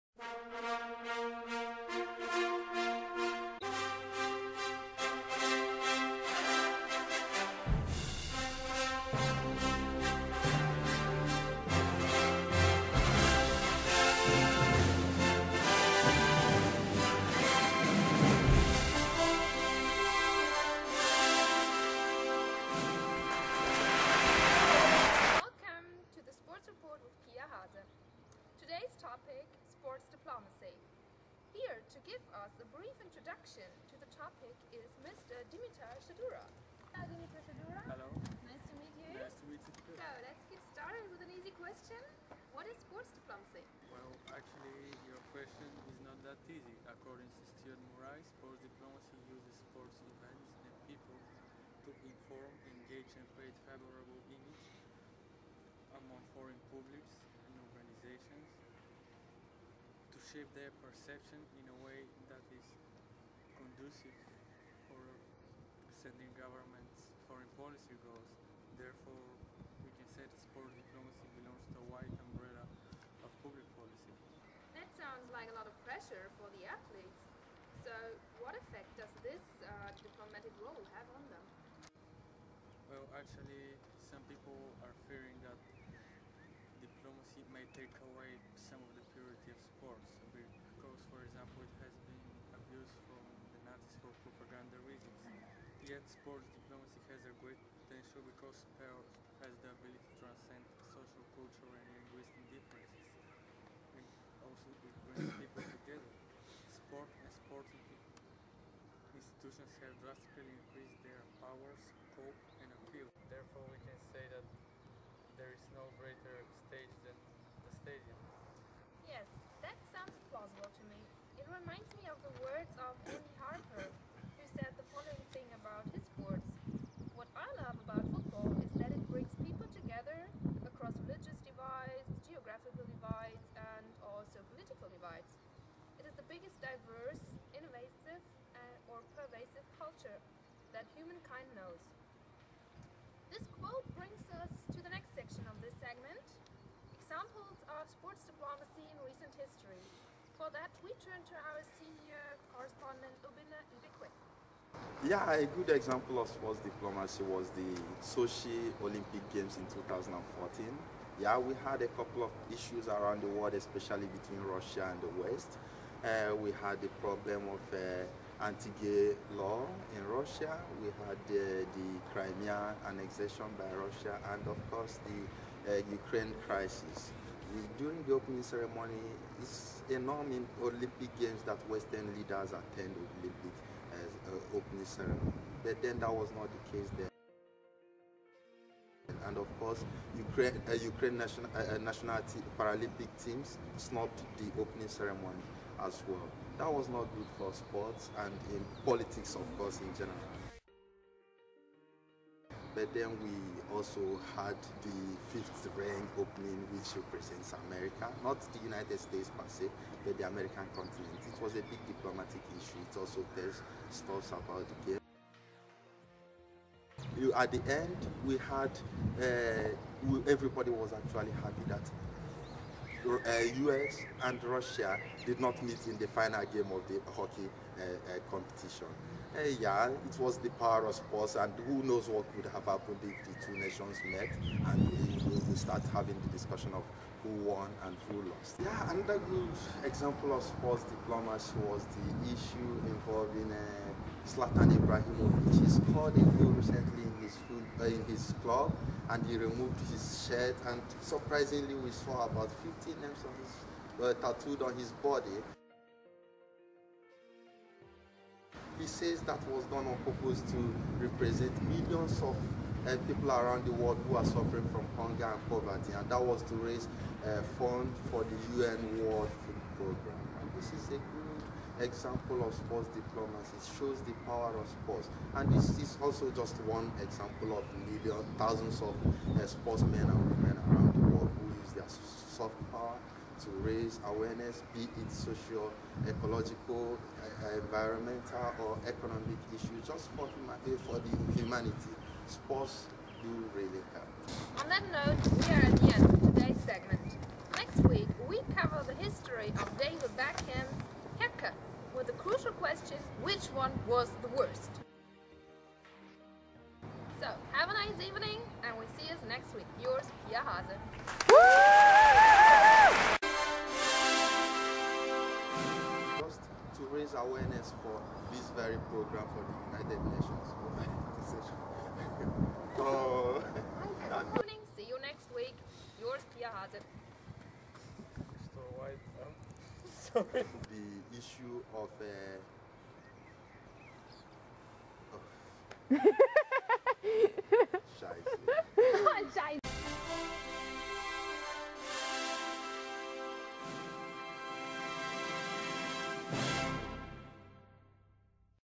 Student-Made Video Project: Sports Diplomacy
A student-made video on sports diplomacy for the class European Diplomacy and Foreign Policy 2015.